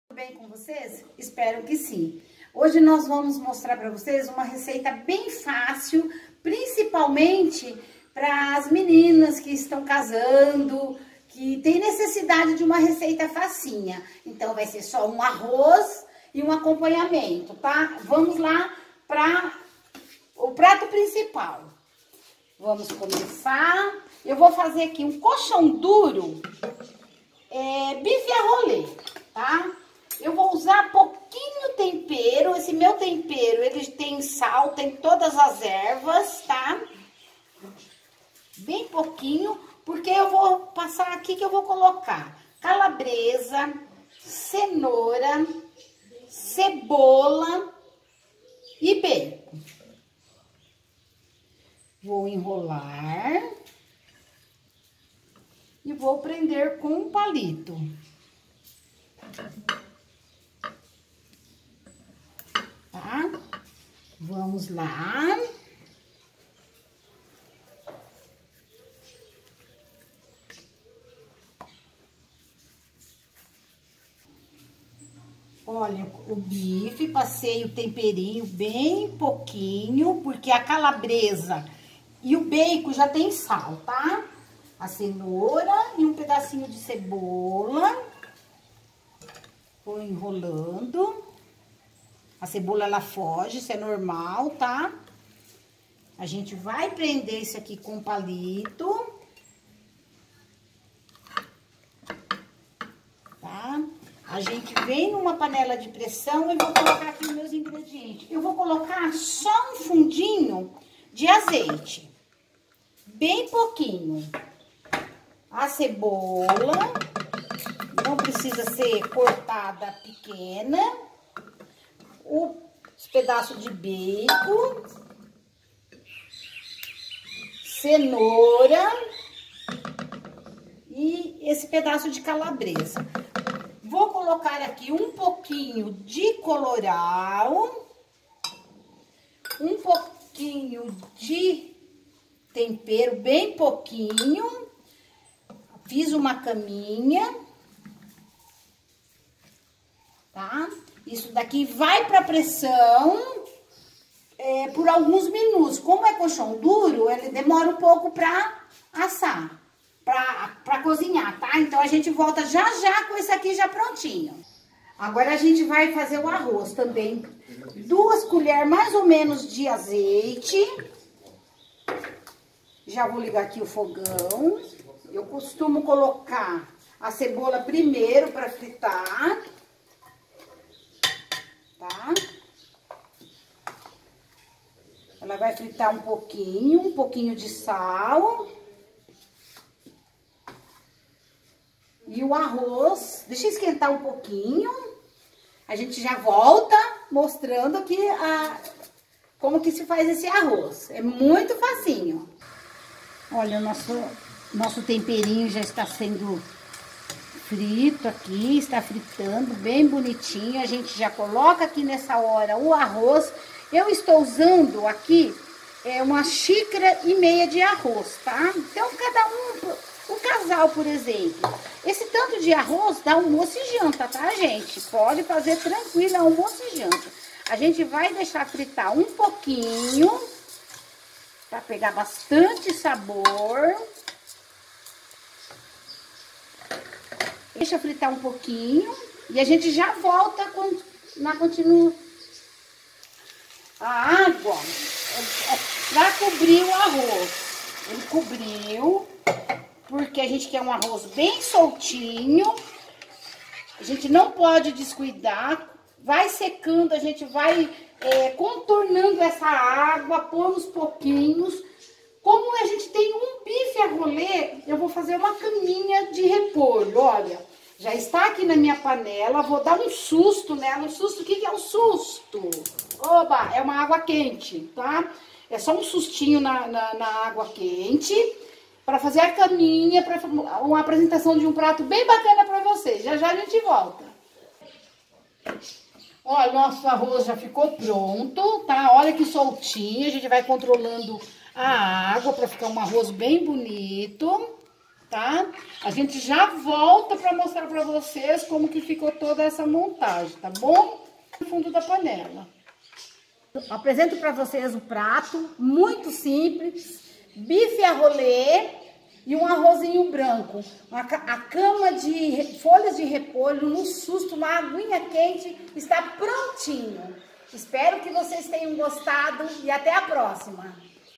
JANTAR PARA CASAL! RECEITA FÁCIL E PRÁTICA, LEIA A DESCRIÇÃO DO VÍDEO PARA + INFORMAÇÕES.
Olá meninos (as) tudo bem?! Fiz um passo a passo de um jantar muito gostoso para recém casados, ideal para quem não tem muita experiência na cozinha!

Ingredientes para preparar a carne:
- 700 g de coxão duro;
-1/2 colher de sopa de tempero (da sua preferência);
-150 gramas de bacon 9 cortado em cubos);
-1 cenoura média;
- 1 cebola;
- 1 calabresa;
- Palitos de dente ( para enrolar o bife);
- Azeite e Colorau;
500ml de água na panela de pressão (5min na pressão).

Ingredientes arroz:

- 2 colher de sopa de Azeite;
- 1/2 cebola picada;
- 1/2 colher de sopa de tempero;
- 1 e 1/2 de Arroz;
Água para cobrir.

Repolho:

07 folhas de repolho e água para assustar!!